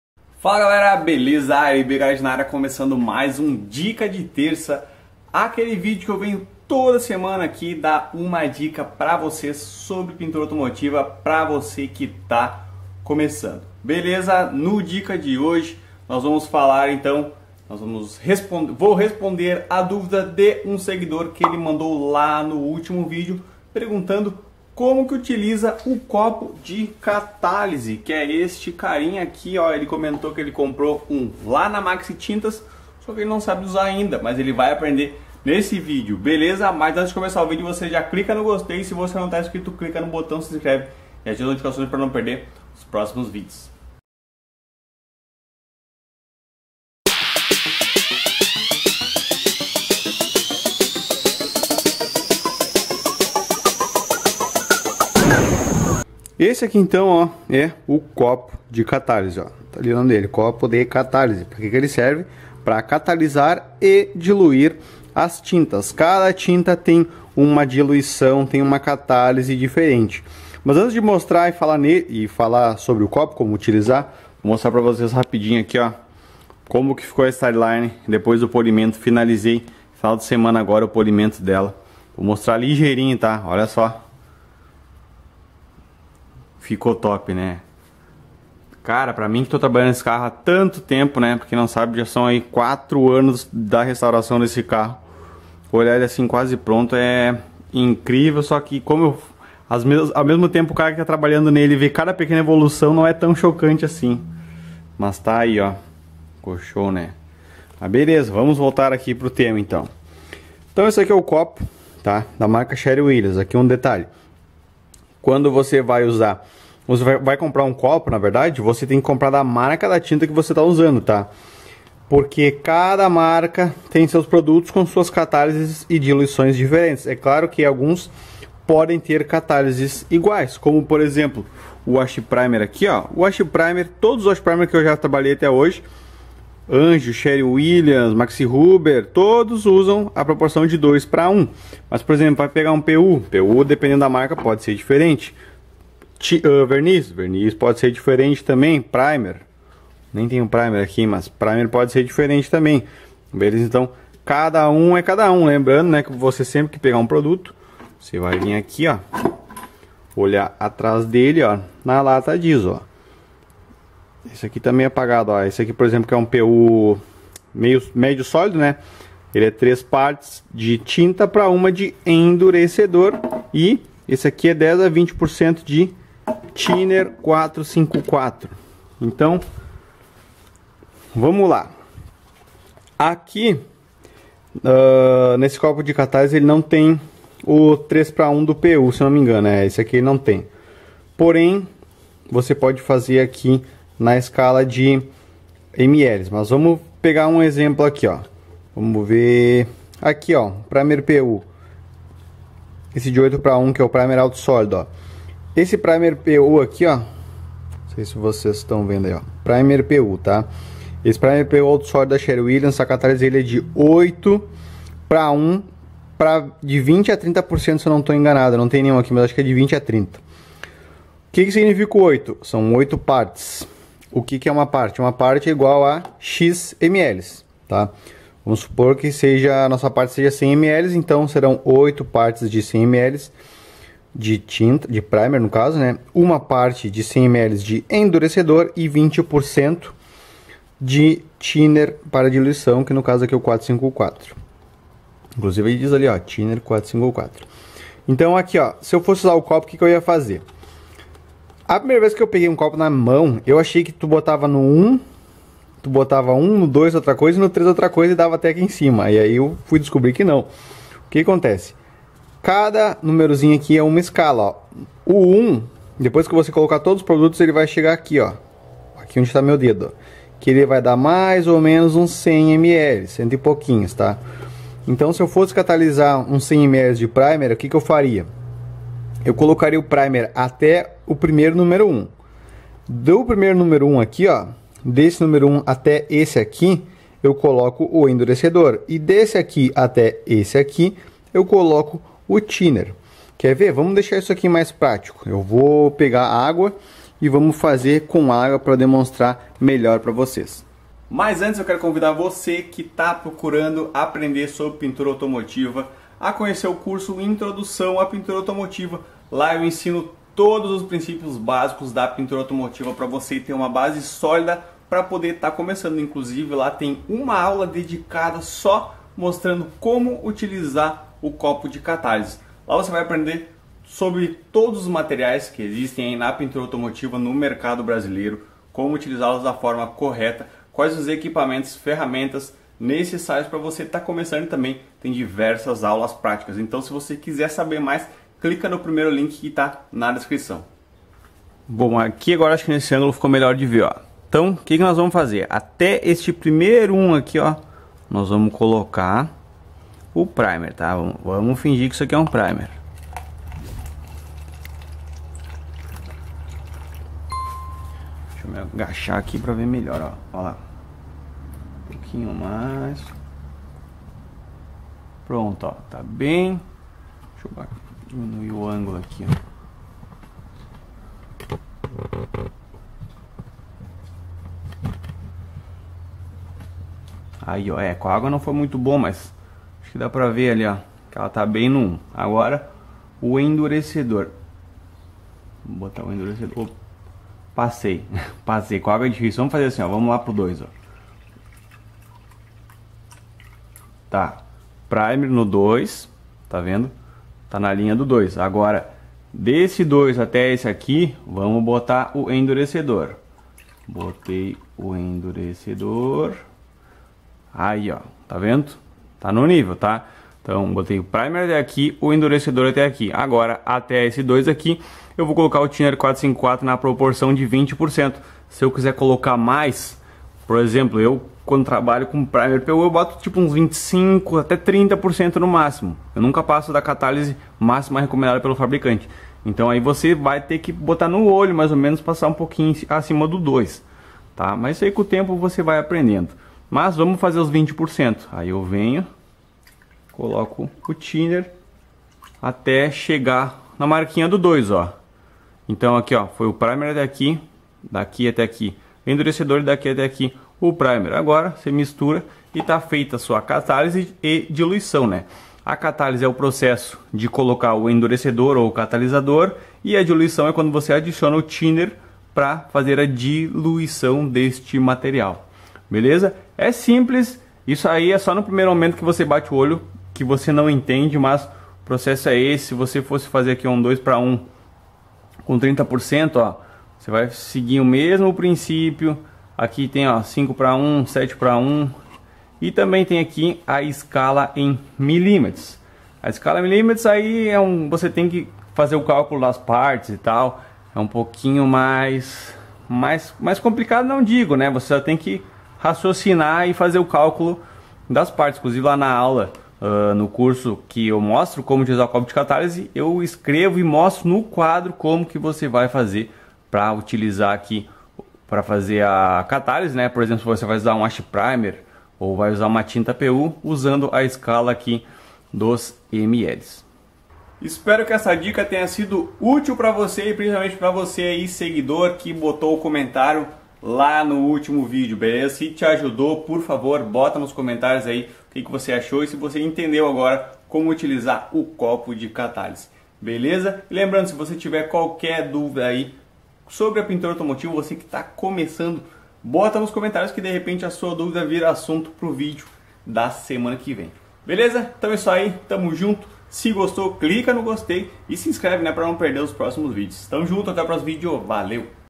COMO USAR COPO DE CATÁLISE NA PINTURA AUTOMOTIVA [Dica de Terça]
Passo a passo pra você aprender de uma vez por todas como usar o Copo de Catálise para catalisar e diluir suas tintas da forma correta e evitar problemas na sua pintura!

CAPÍTULOS DESSE VÍDEO:
00:00 Intro
00:55 Copo de Catálise pra que serve?
05:41 Catálise na prática